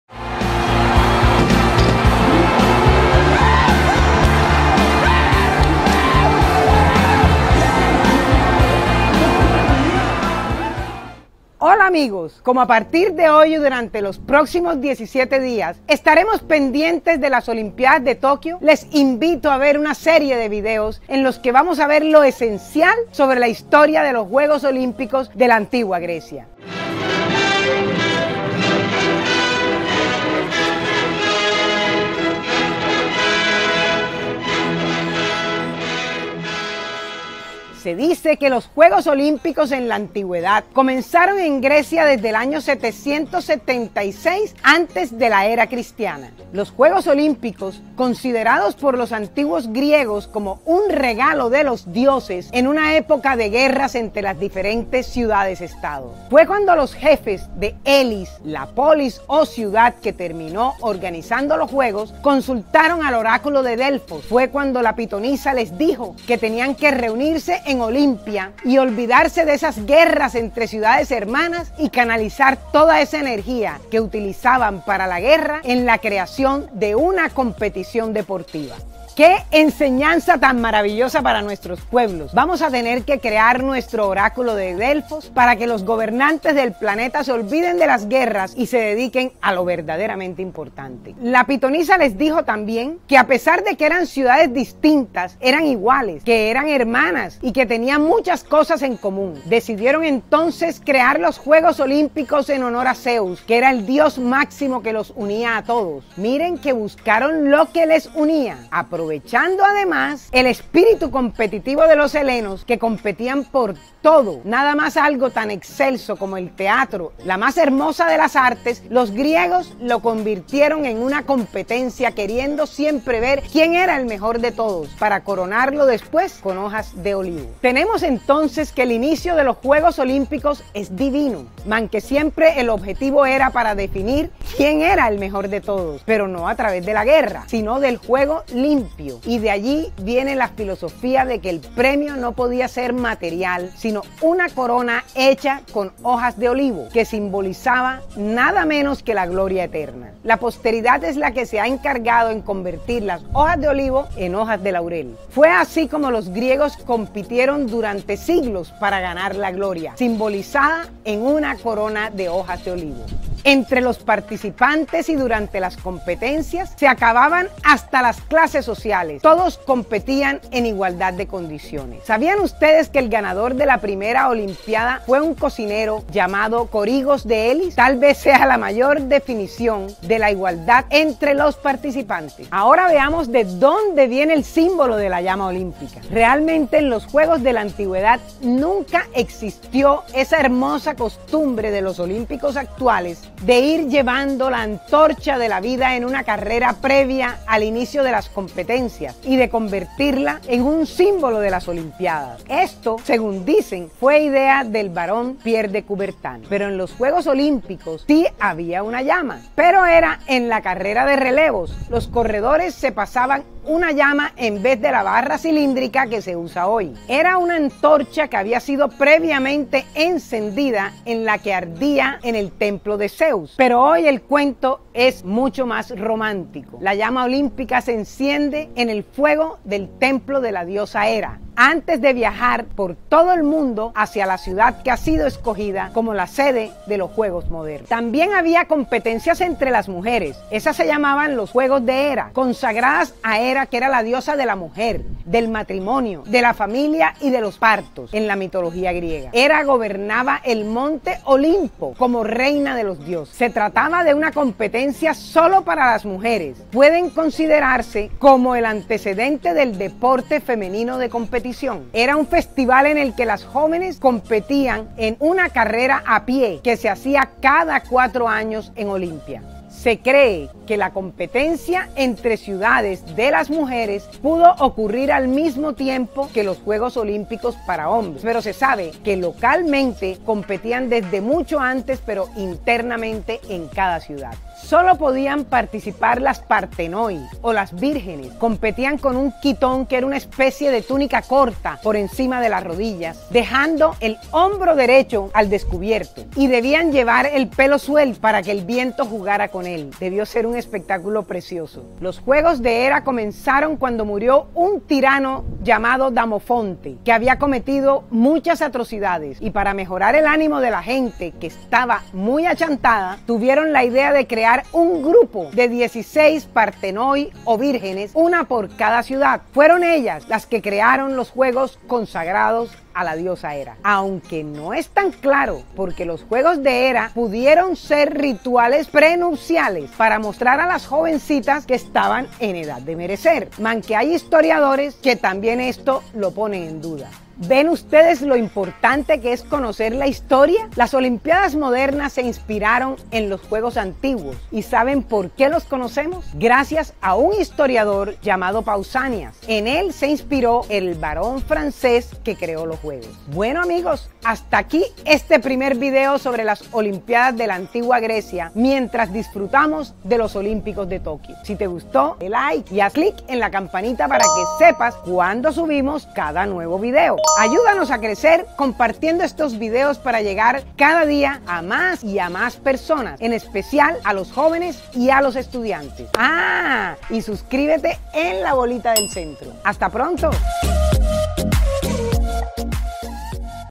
La historia de las Olimpiadas de la Antigua Grecia: origen, la llama olímpica y los juegos de Hera
En este video, nos sumergiremos en la historia de las Olimpiadas de la Antigua Grecia, descubriendo sus orígenes, la llama olímpica y los juegos de Hera.

Acompáñanos en un viaje en el tiempo para explorar el nacimiento de las Olimpiadas en la antigua Grecia y su evolución a lo largo de los siglos.

Créditos:
Dirección, investigación, guión y presentación: Adelina Covo
Producción y edición: Antonio Luis Ángel Covo

Fuentes:

 - Briceño Jáuregui, Manuel, S.J. Los juegos olímpicos en la antigüedad. Intituto Caro y Cuervo. Bogotá.1990

Consultado el 210721

 - Pausanias. Descripción de Grecia. Tomo 3, Libro 5: Elide. Capitulo 4, 5 – 6.

 - Blog AH Web. 104º Olimpíada, 364 a.C.: Elis vs Pisa. Publicado el 15 de agosto de 2012. Disponible en:
Consultado el 210720